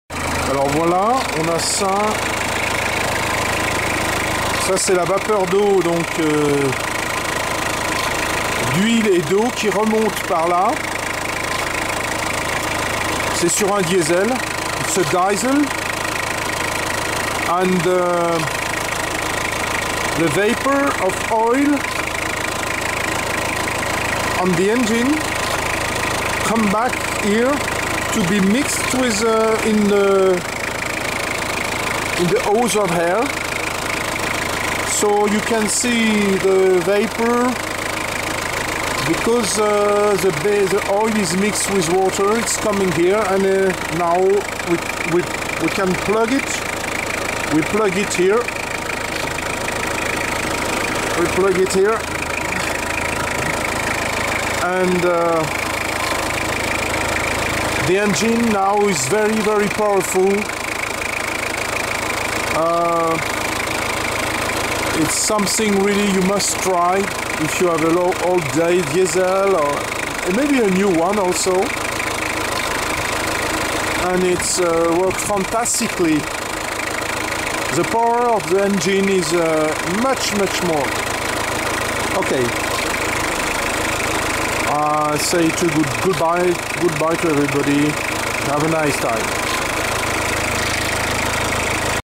Diesel et vapeur d'eau, Diesel and water vapor
Mixing a little quantity of cooling liquid into the engine oil, you will create water vapor that will mixed with diesel vapor as it comes up
into the air hose, existing  in every diesel engine. I do not realy know what will happen in modern diesel, so much tricks that are put around the engine (inefficient, like catalytic converter, only there to replace hidding efficient technologies, and make sure you and ensure that you continue to consume unnecessary fuel)